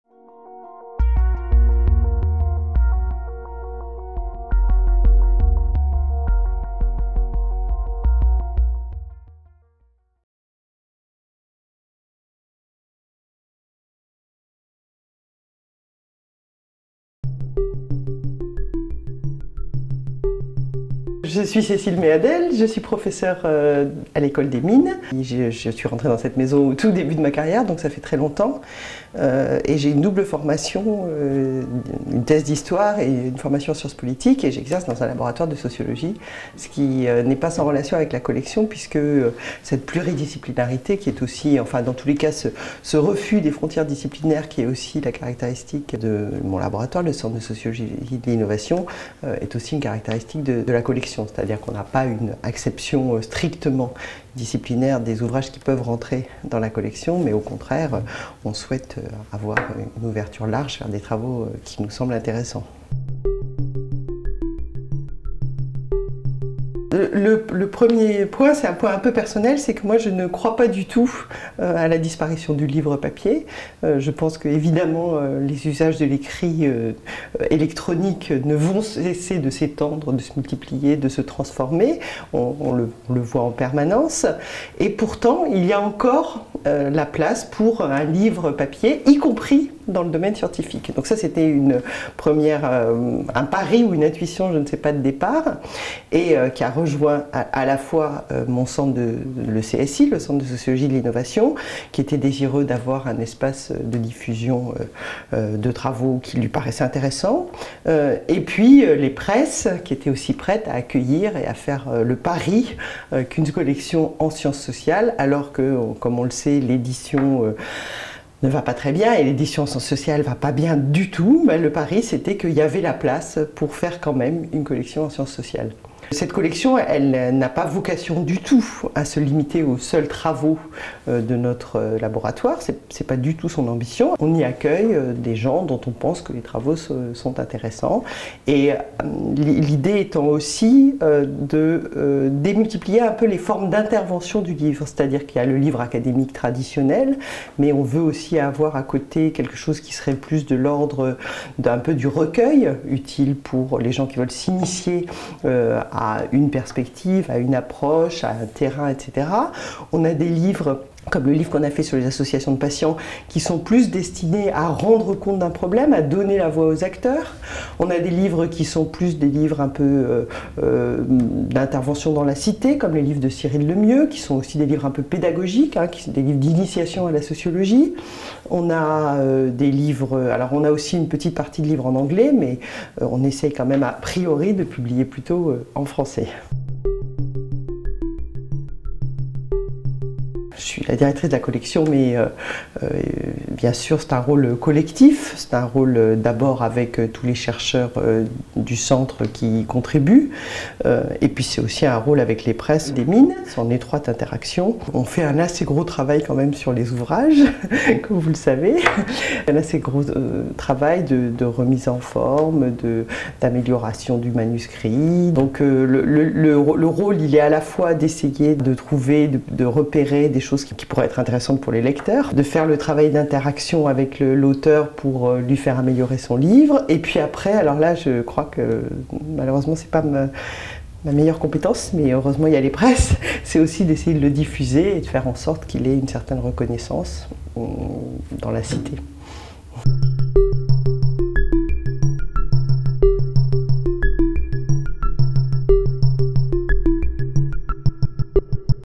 Interview de Cécile Méadel
Cécile Méadel, sociologue au CSI (centre de sociologie de l'innovation) et professeure à Mines ParisTech , présente la collection Sciences sociales qu'elle dirige aux Presses des Mines et nous parle de l'importance de sauvegarder le livre papier en parallèle  de la publication numérique.